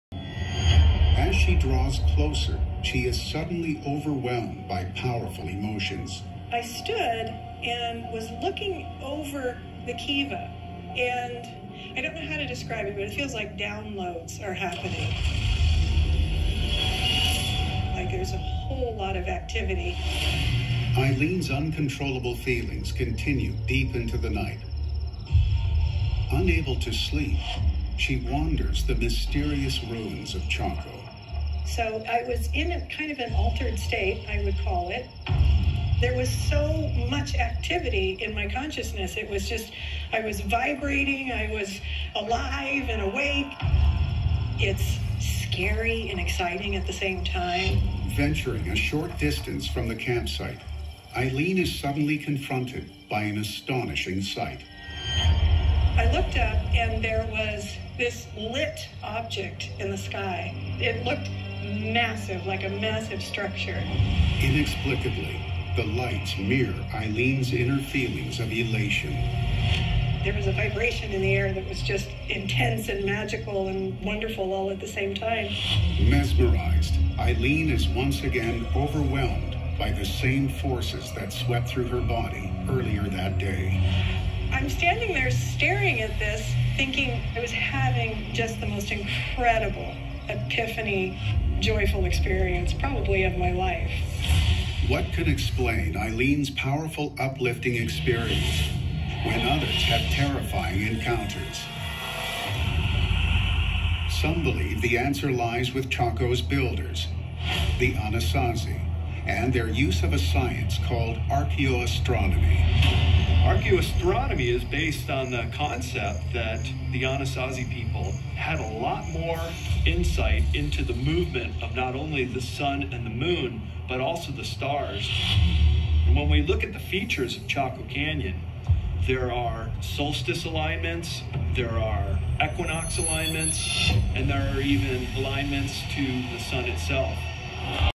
Chaco Canyon: Time Traveling Portal?
A TV producer read my 2013 blog. She was researching their planned episode about Mysteries in the National Parks Secrets and Legends: Chaco Canyon. Mainly, they wanted to explore why some people had horrifying experiences, while others not only embraced but were fed by the intensity in this locale. It was a great experience, and they got the bits and pieces they needed to weave together an interesting show. But my story was much larger. I'll fill you in on the next podcast episode - all that was left on the cutting room floor.

Chaco Canyon is the topic of my next podcast/audiobook, Ep. 8, in a few weeks. Leading up to this 2013 trip there was an invitation to come to Chaco via download, along with visitations, a lucid dream of a lion-being demonstrating time travel in the kiva, & more. In early 2022, a new invite was received. Episode 8 is the backstory. Episode 9 will bring us closer to the present.

Link to the television show, National Parks Secrets and Legends, Season 2/Episode 7